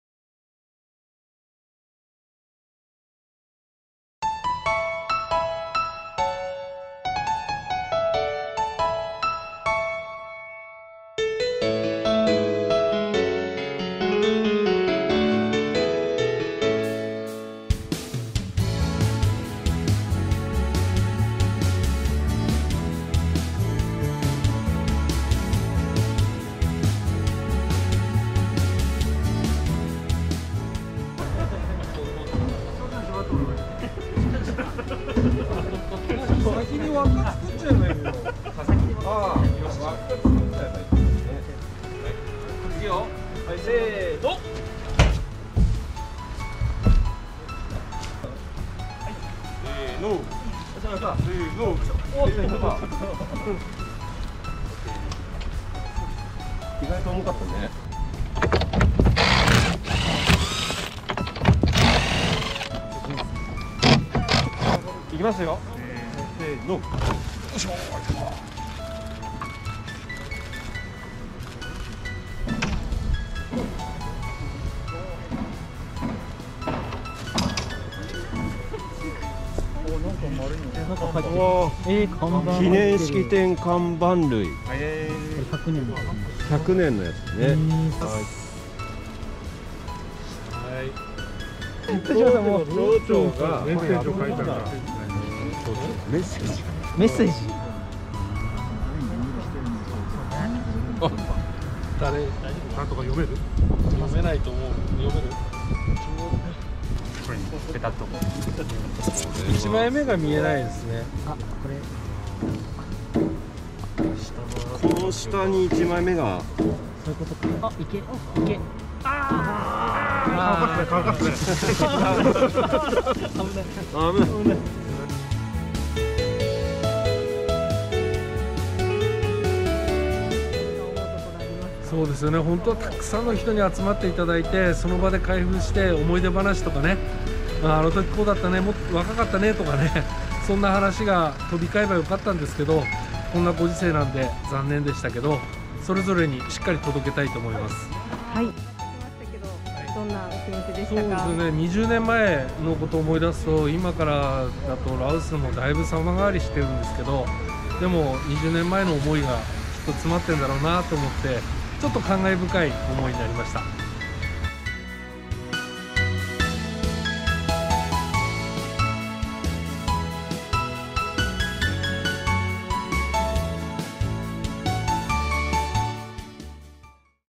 タイムカプセル～引き上げ・開封作業～
2021年10月19日、タイムカプセルの引き上げ・開封作業を行いました。

埋蔵日は2000年9月19日。
356人の思いを込めたタイムカプセルが21年と1ヶ月の時を越えて、引き上げられました。